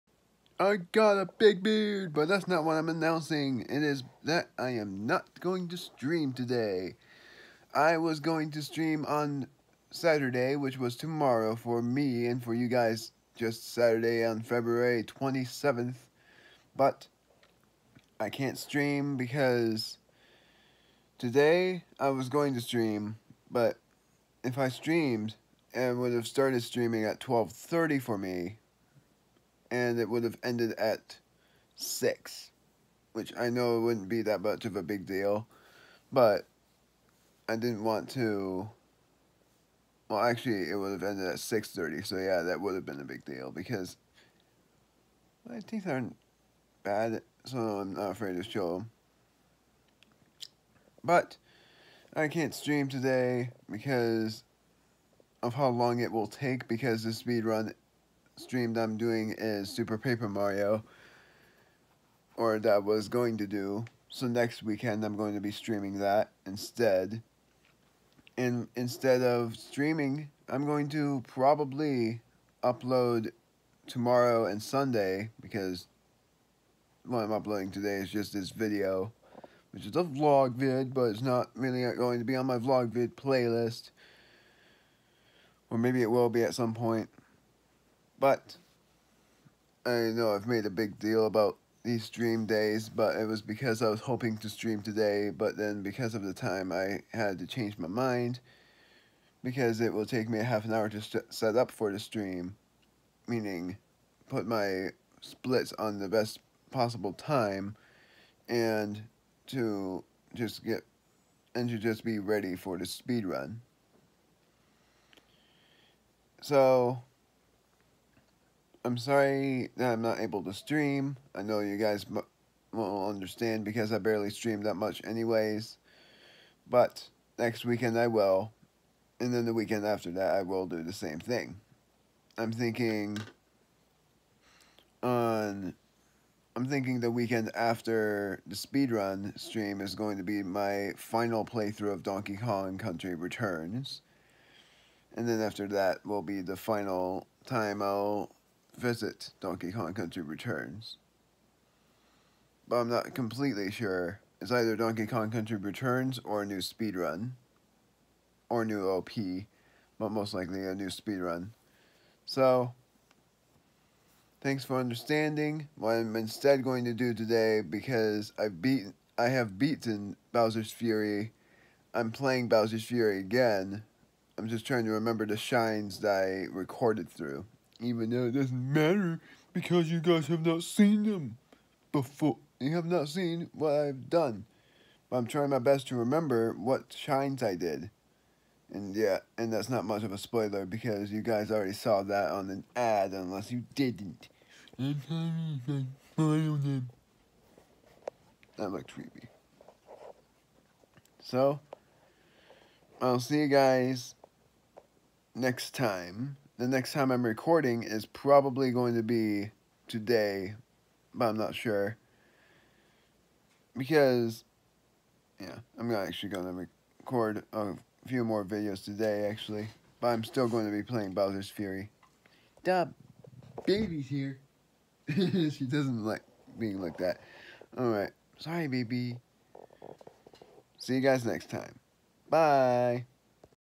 Can’t Stream This Weekend (February 26-28)
I can’t stream this weekend because I have things planned, but thankfully I will be able to next weekend. I am going to record though, but they are the final videos of Super Mario 3D World.